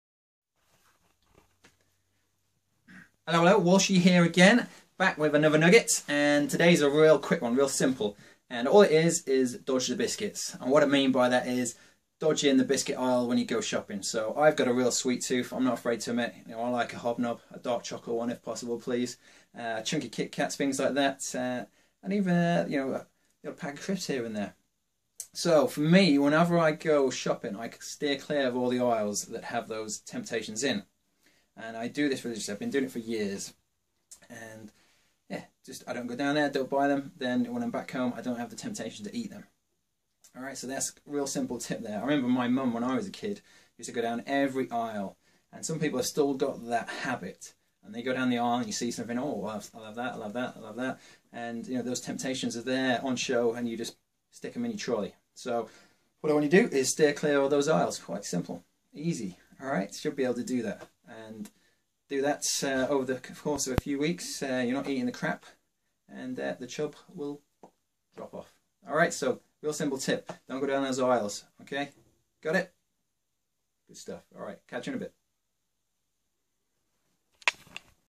Fat Loss Nuggets #5: Aisle dodging
- Walshy talks about his super market tactics.